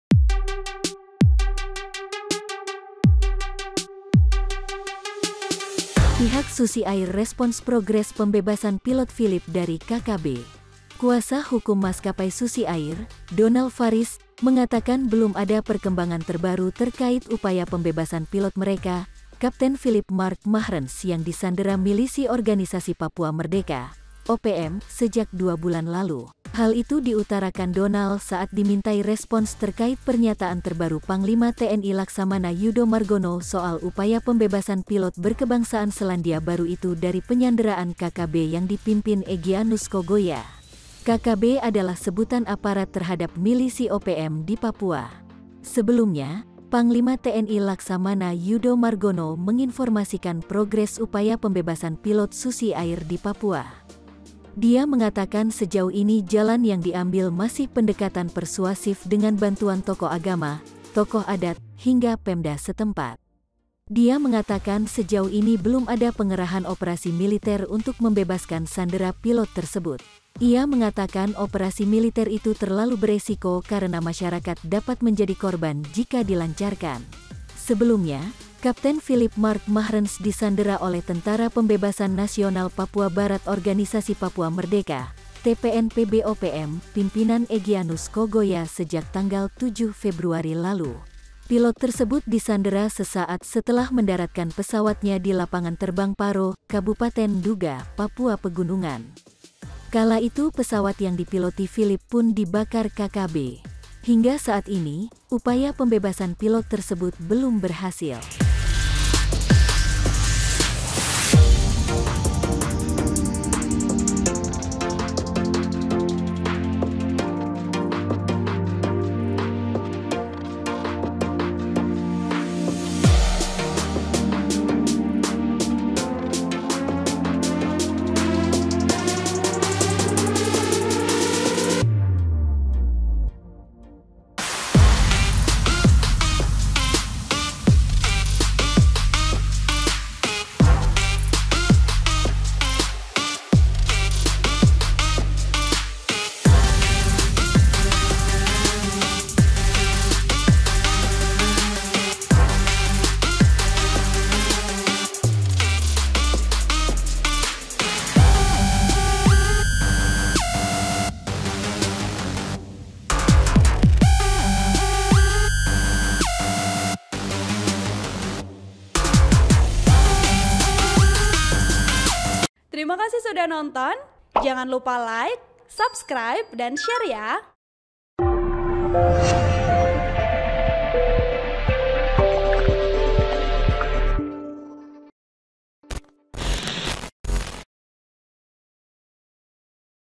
Pihak Susi Air Respons Progres Pembebasan Pilot Philip dari KKB
POS-KUPANG.COM - Kuasa hukum maskapai Susi Air, Donal Fariz, mengatakan belum ada perkembangan terbaru terkait upaya pembebasan pilot mereka, Kapten Philip Mark Mahrtens yang disandera milisi Organisasi Papua Merdeka sejak dua bulan lalu.

Hal itu diutarakan Donal saat dimintai respons terkait pernyataan terbaru Panglima TNI Laksamana Yudo Margono soal upaya pembebasan pilot berkebangsaan Selandia Baru itu dari penyanderaan KKB yang dipimpin Egianus Kogoya.

KKB adalah sebutan aparat terhadap milisi OPM di Papua.

Sebelumnya, Panglima TNI Laksamana Yudo Margono menginformasikan progres upaya pembebasan pilot Susi Air di Papua. Dia mengatakan sejauh ini jalan yang diambil masih pendekatan persuasif dengan bantuan tokoh agama, tokoh adat, hingga pemda setempat. Dia mengatakan sejauh ini belum ada pengerahan operasi militer untuk membebaskan sandera pilot tersebut.

Ia mengatakan operasi militer itu terlalu beresiko karena masyarakat dapat menjadi korban jika dilancarkan.

Sebelumnya, Kapten Philip Mark Mahrtens disandera oleh Tentara Pembebasan Nasional Papua Barat Organisasi Papua Merdeka (TPNPB-OPM) pimpinan Egianus Kogoya sejak 7 Februari lalu.
(POS-KUPANG.COM-Lenny)